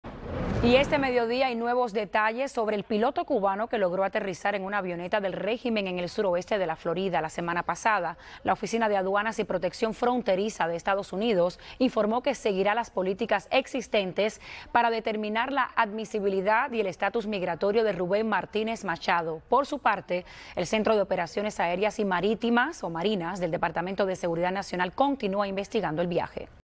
Determinan estadía de piloto cubano que llegó a Florida en avión ruso
Una nave del régimen cubano fue desviada este viernes hacia el Sur de la Florida y, en tiempo récord, su piloto, identificado como Rubén Martinez de 29 años, llegó al aeropuerto de Entrenamiento Dade -Collier en Ochopee, al oeste del condado. El piloto dijo que había volado desde Sancti Spiritus, en Cuba.